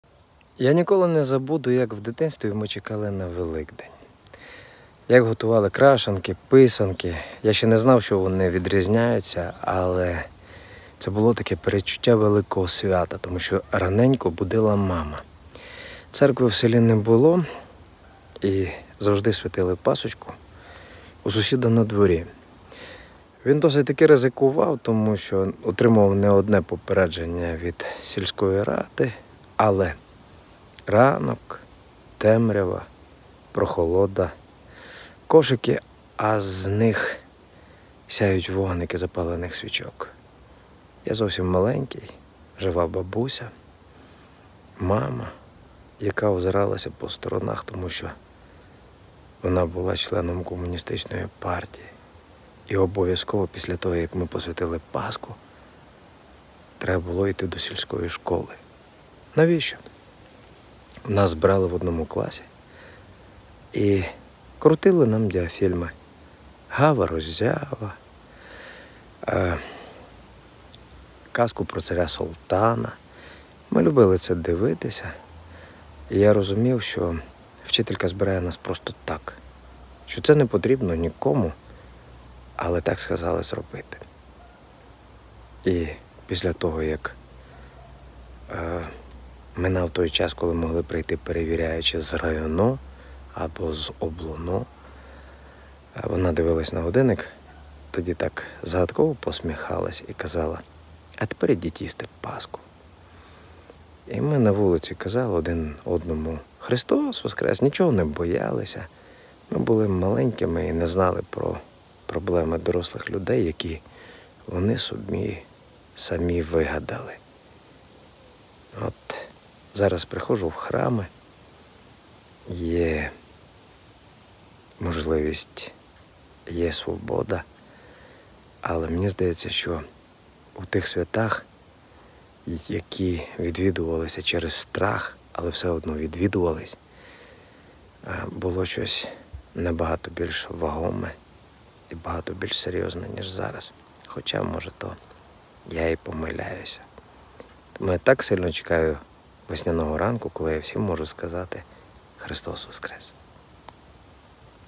Петро Мага - Зірки про улюблені свята
Відомі люди розповідають про улюблені свята.